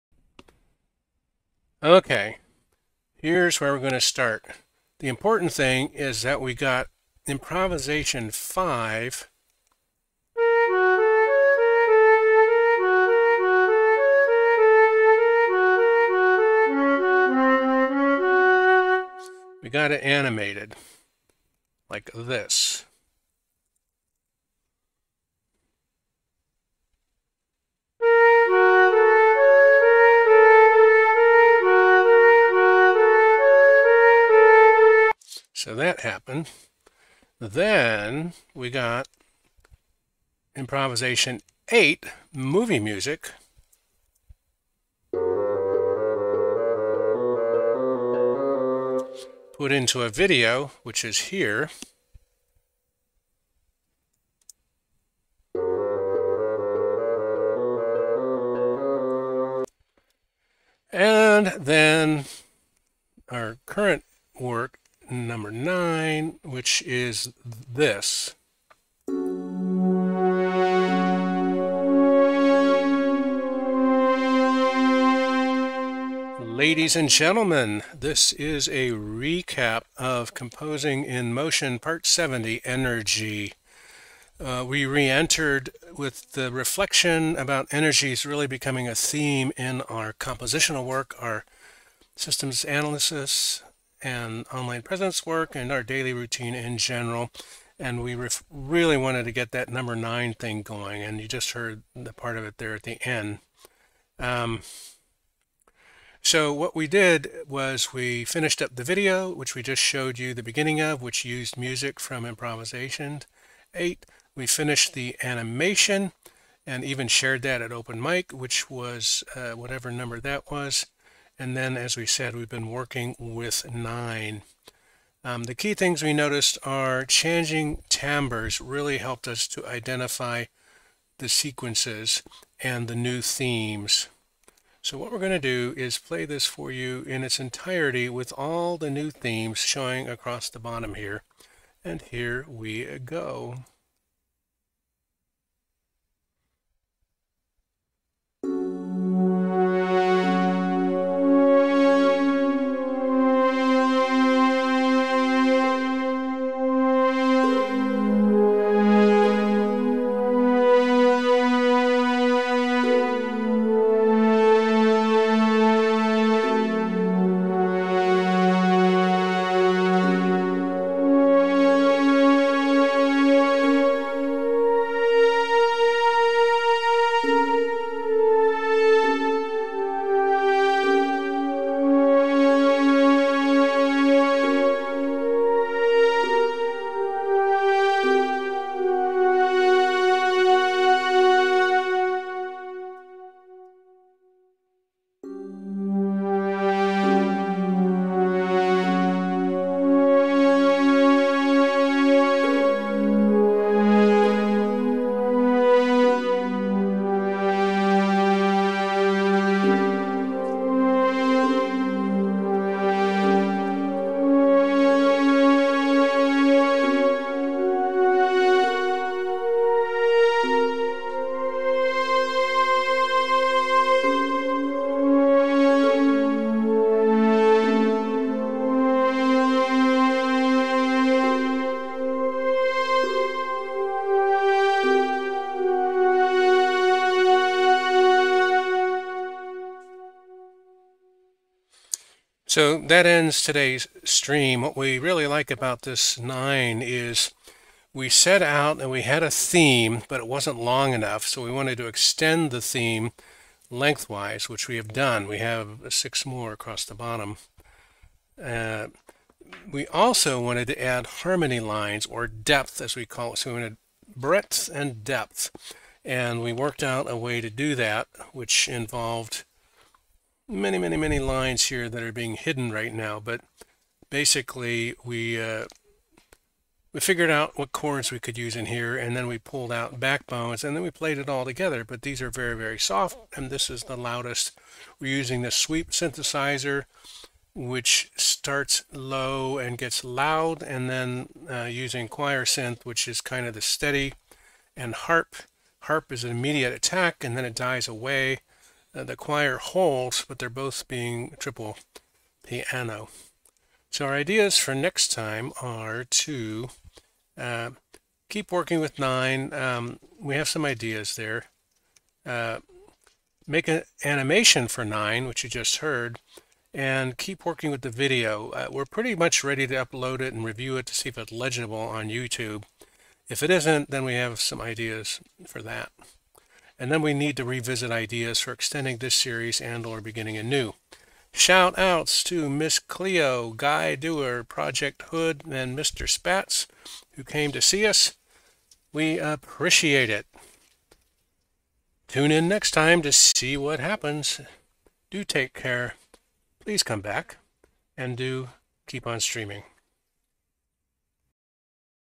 Composing in Motion Part 70 ENERGY Stream Highlight 12 Jul 2021
five animation and eight movie music are completed; composition nine is continued